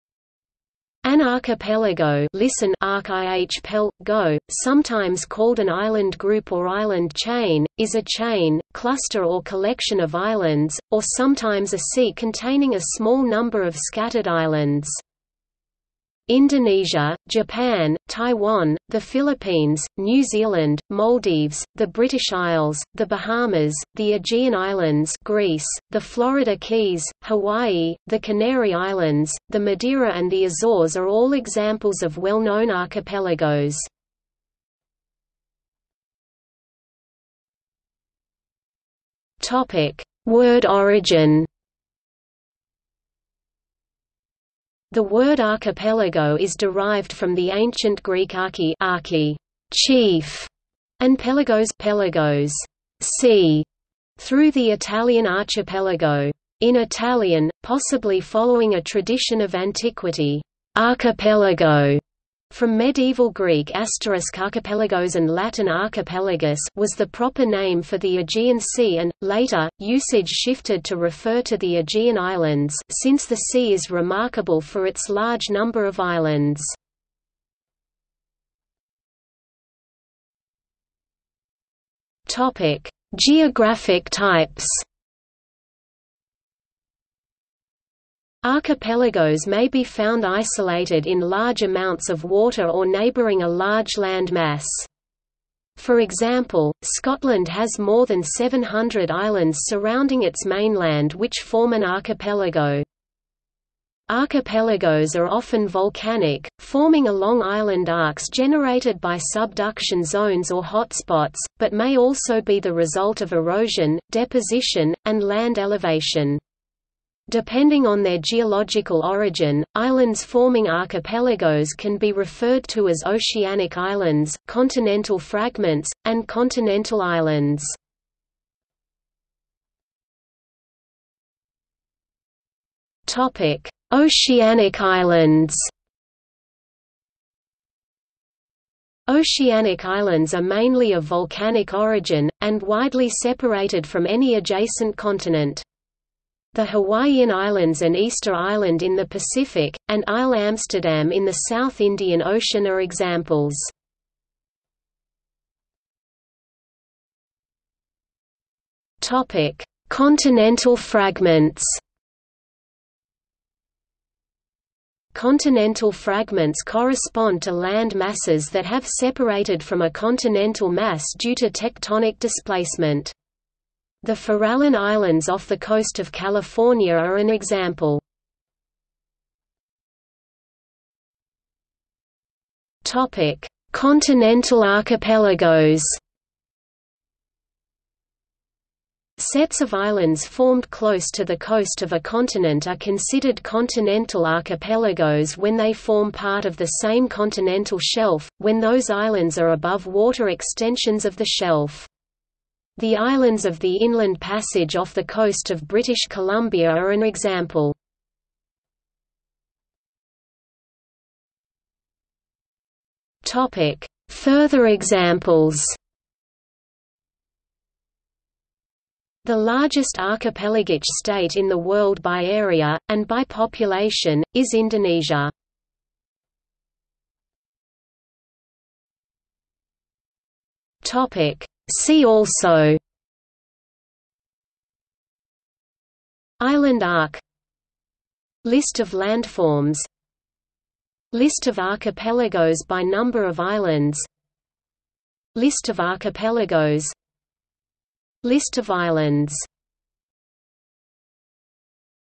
Archipelago | Wikipedia audio article
00:00:35 1 Word origin
00:01:18 2 Geographic types
00:02:09 2.1 Oceanic islands
00:02:35 2.2 Continental fragments
00:02:59 2.3 Continental archipelagos
00:03:29 3 Further examples
00:03:47 4 See also

- Socrates

SUMMARY
An archipelago ( (listen) ARK-ih-PEL-ə-goh), sometimes called an island group or island chain, is a chain, cluster or collection of islands, or sometimes a sea containing a small number of scattered islands.
Indonesia, Japan, Taiwan, the Philippines, New Zealand, Maldives, the British Isles, the Bahamas, the Aegean Islands (Greece), the Florida Keys, Hawaii, the Canary Islands, the Madeira and the Azores are all examples of well-known archipelagos.